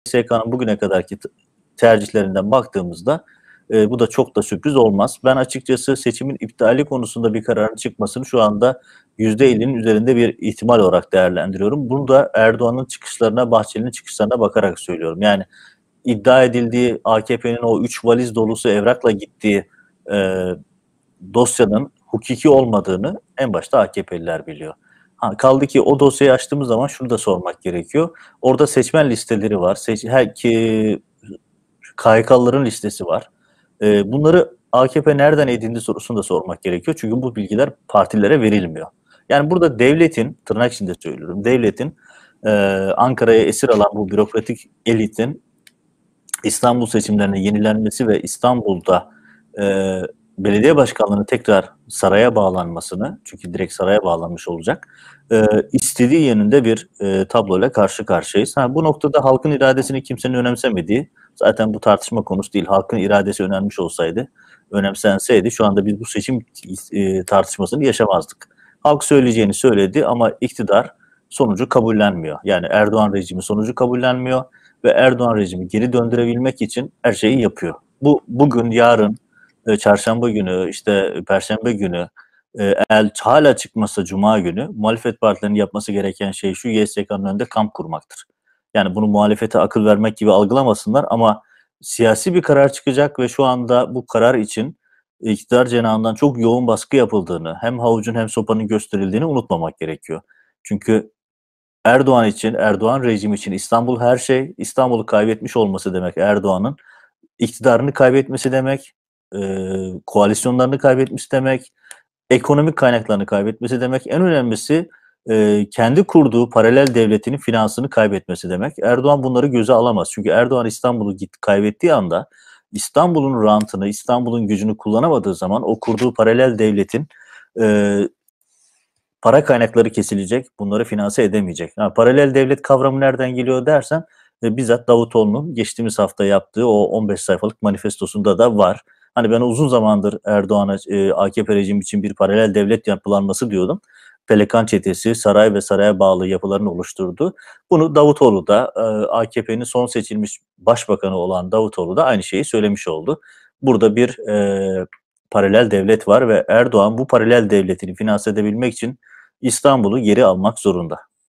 İşte Erdoğan'ın Ekrem İmamoğlu korkusunun asıl sebebi | İstanbul seçim sonuçları kavgası
Cumhurbaşkanı Tayyip Erdoğan Ekrem İmamoğlu'nu devirip İstanbul'u geri almayı neden bu kadar çok istiyor? Gazeteci Adem Yavuz Arslan, AKP'nin YSK'ya İstanbul seçiminin iptali için yaptığı baskının perde arkasını anlattı. Arslan, ''Eğer Erdoğan İstanbul'u kaybederse devlet içinde kurduğu paralel yapıyı beslemeyez ve iktidarı kaybeder'' dedi.